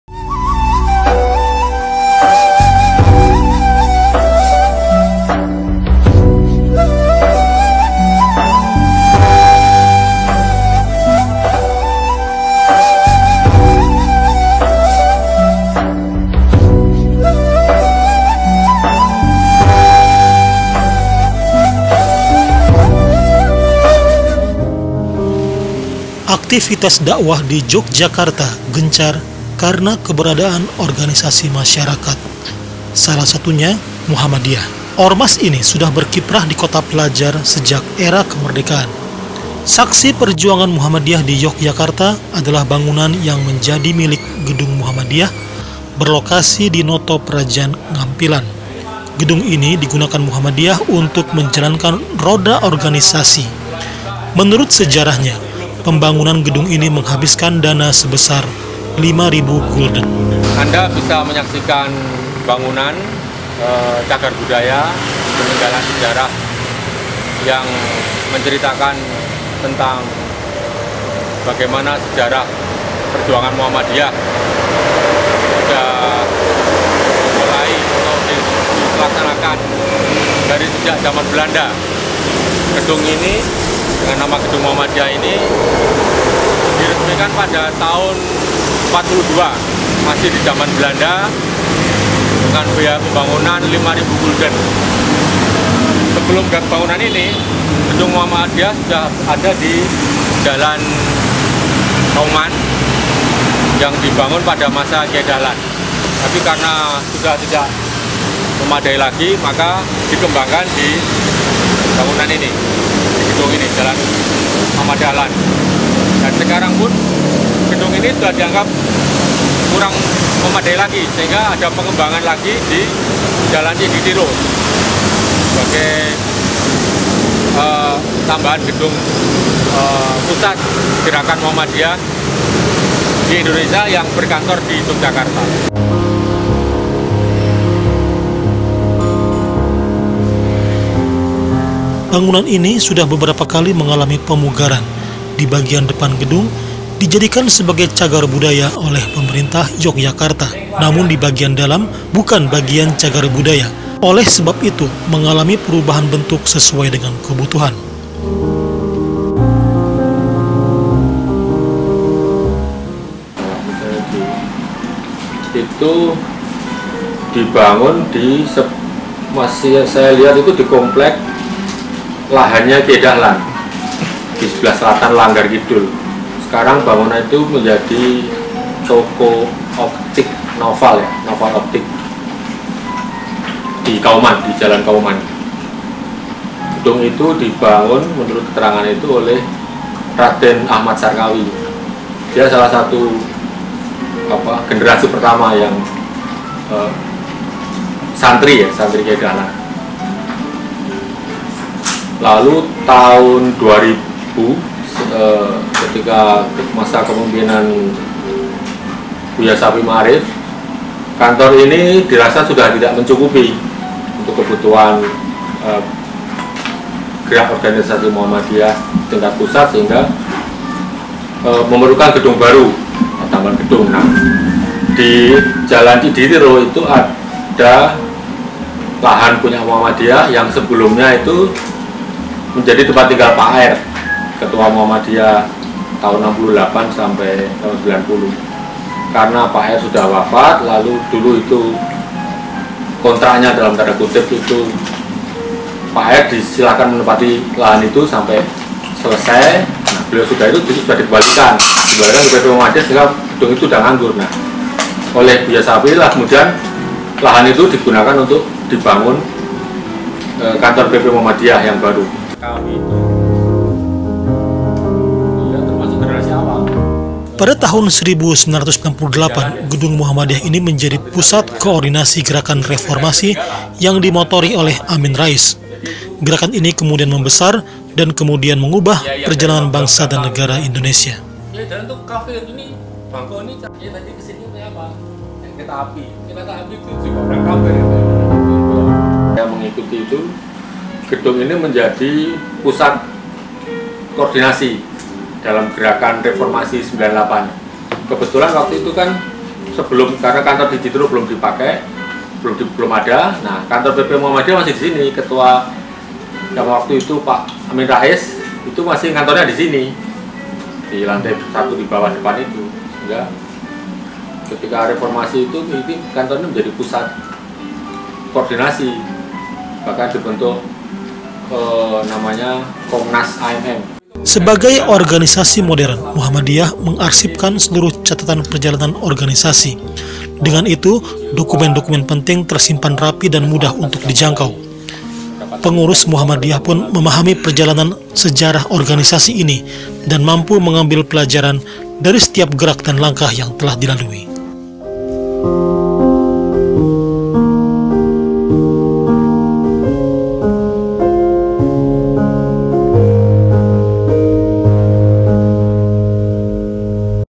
SEJARAH MUHAMMADIYAH: GEDUNG YANG DIBANGUN LIMA RIBU GULDEN
Gerak langkah Muhammadiyah sebagai organisasi tidak diragukan lagi. Yogyakarta adalah salah satu saksi bagaimana organisasi ini menghimpun jaringan dalam langkah bangsa dan negara.

Sebuah gedung yang dibangun dengan 5.000 Gulden, menjadi saksi para pengurus Muhammadiyah membangun jaringan dan kerja nyata untuk negeri.

@2019, Media Club Indonesia - Dokumenter Islam di Indonesia